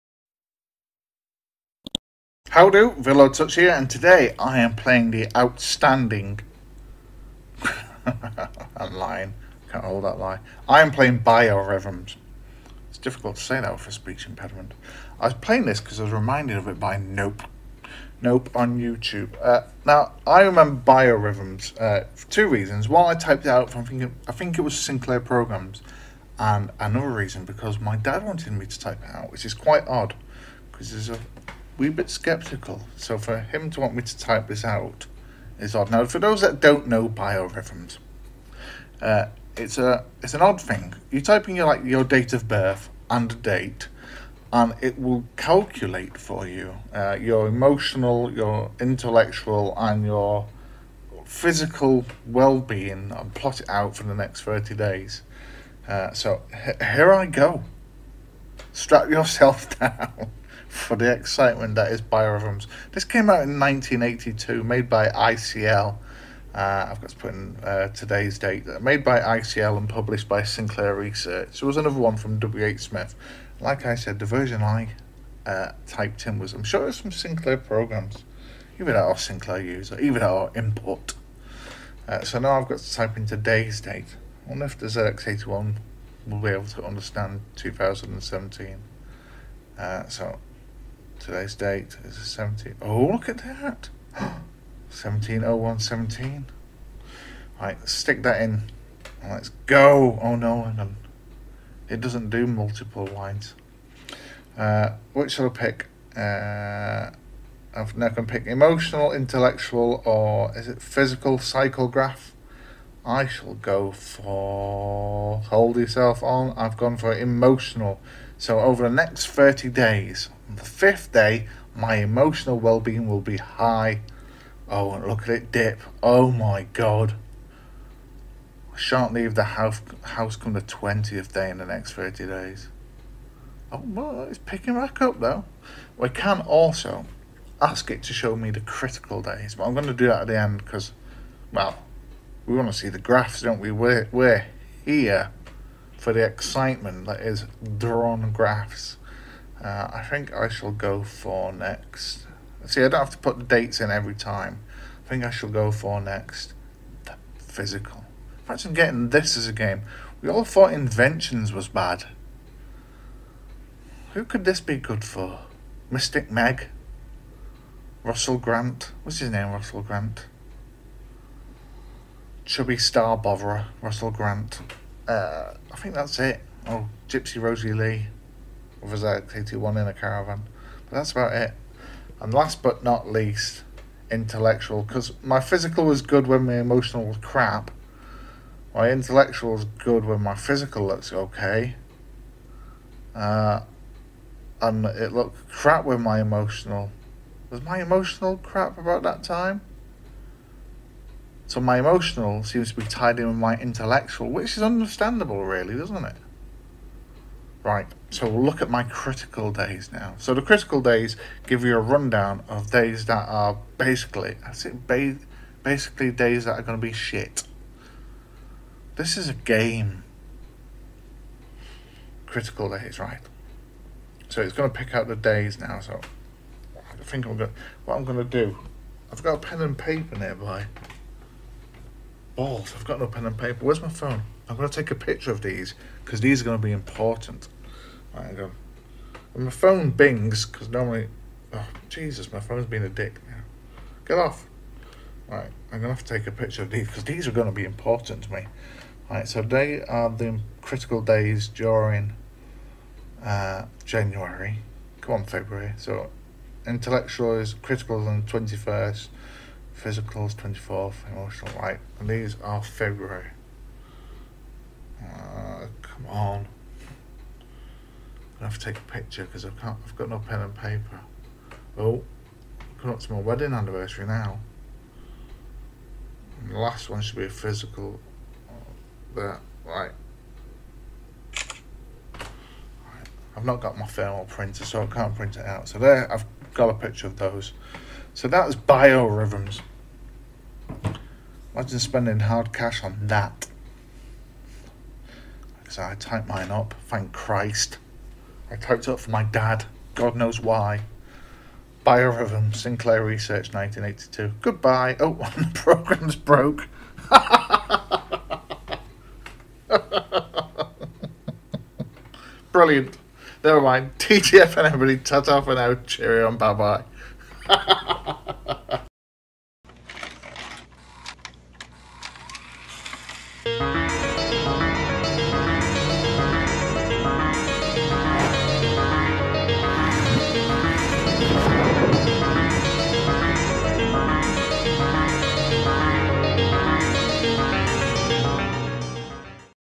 Biorhythms - Learn your future from the humble ZX81
Biorhythms was published for the ZX81 by Sinclair Research in 1982

Created by ICL

Chapters
00:00 Intro
00:12 Loading Screen
00:57 My birthday
01:57 Emotional Graph
02:36 Physical Graph
03:01 Intellectual Graph
03:40 Critical Days
04:47 The end

If you don't have a ZX81 but would like to give Biorhythms a go then download EightyOne (ZX81 Emulator) for free